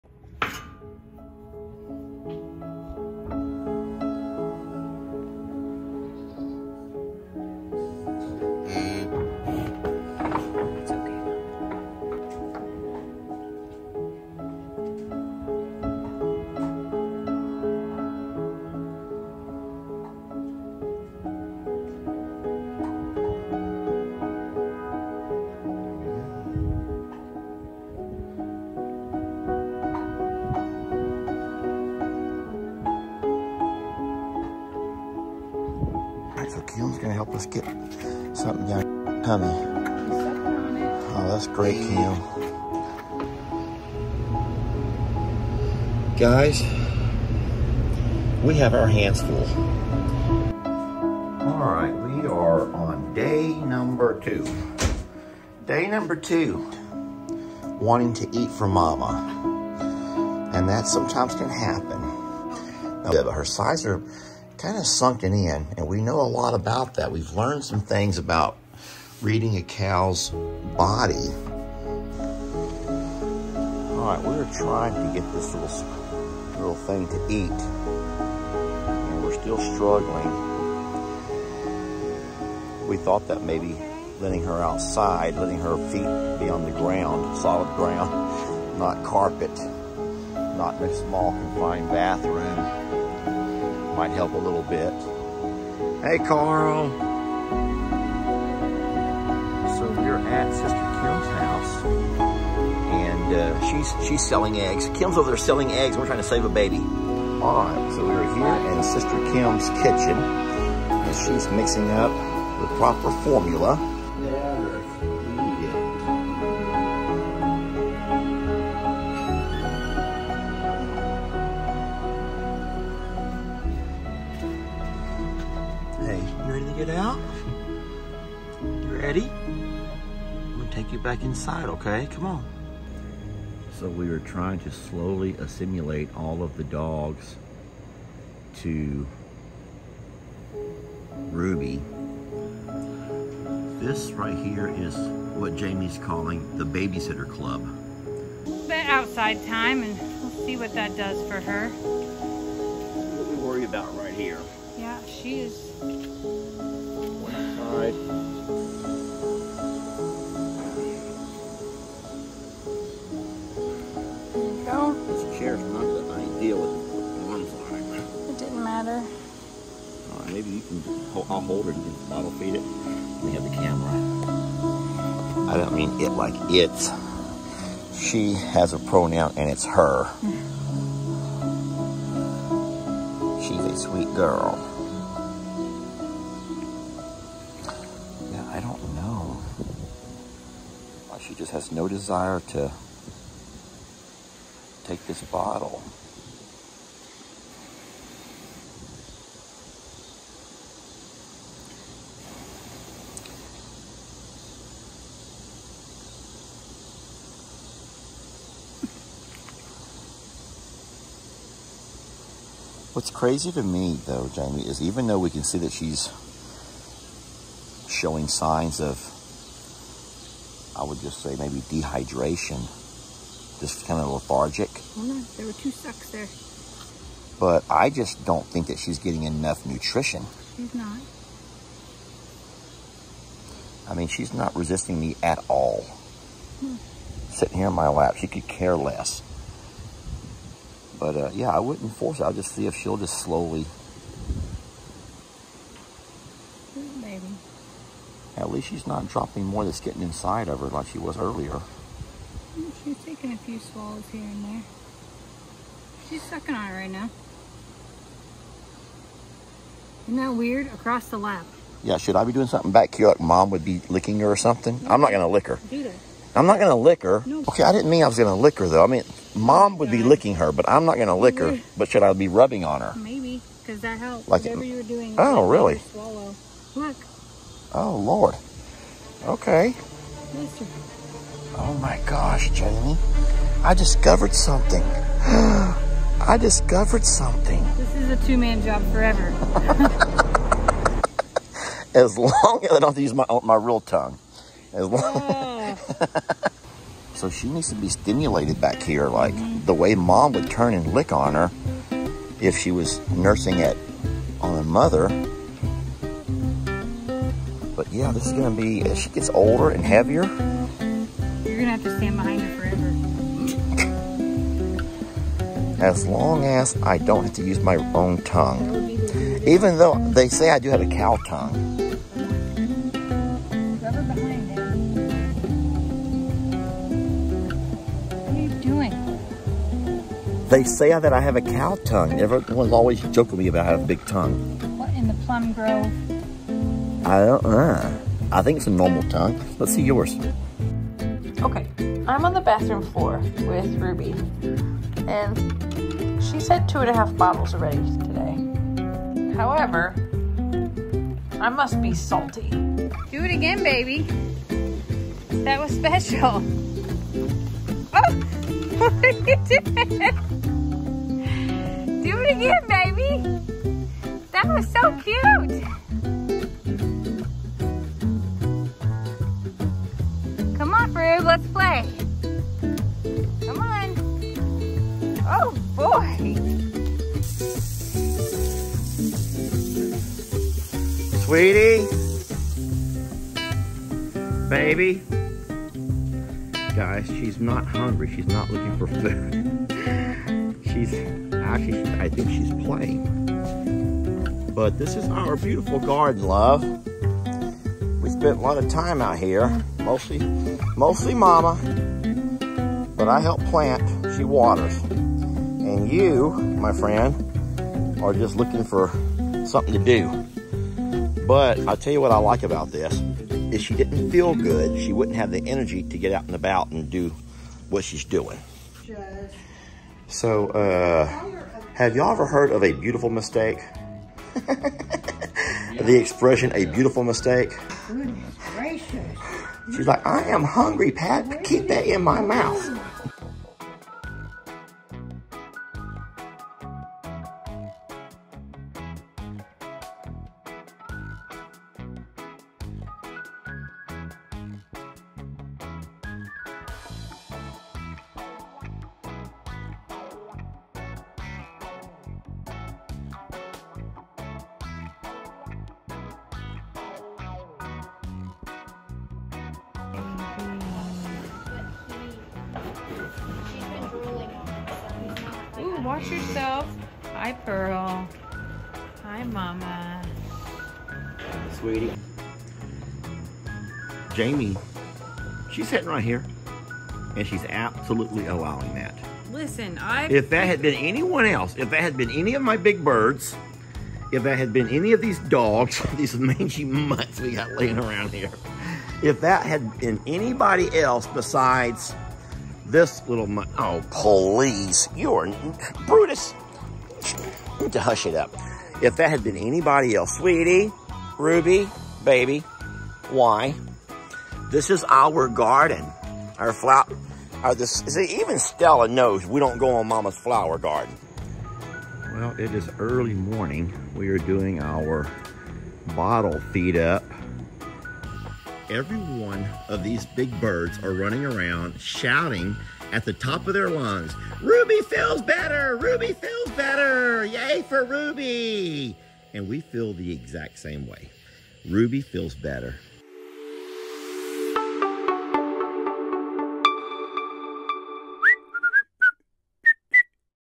Ruby: The Little Longhorn
This little lady has had a rough week but we feel like she has brighter days ahead